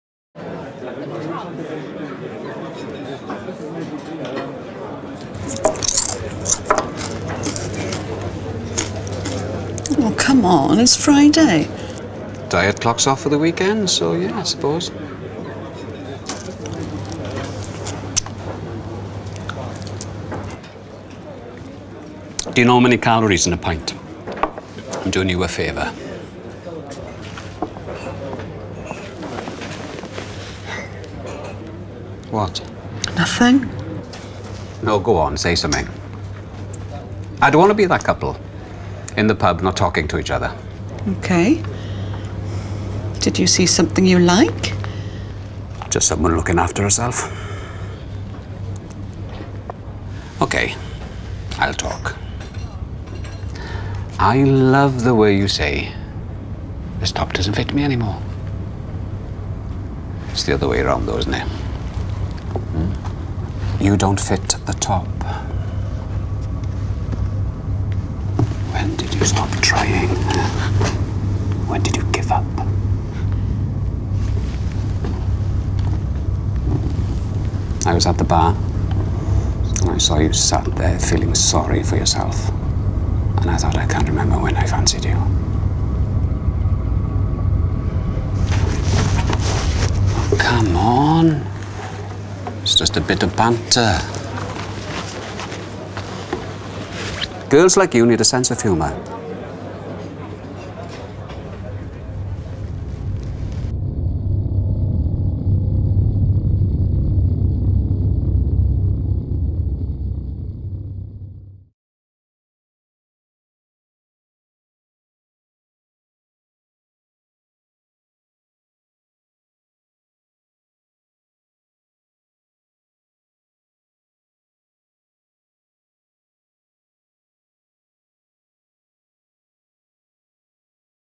Whereistheline - Banter
There’s a thin line between a caring partner and a controlling one. It can be hard to know where that line is, and even harder to know the right thing to do when you see it being crossed.